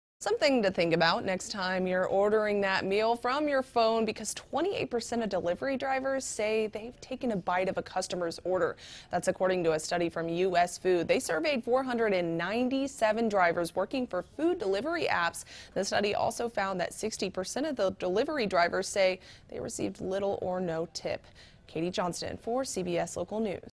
Yuck! Study Says 28 Percent Of Delivery Drivers Taste Your Food
Some food for thought the next time you order a meal from a delivery app. Katie Johnston reports.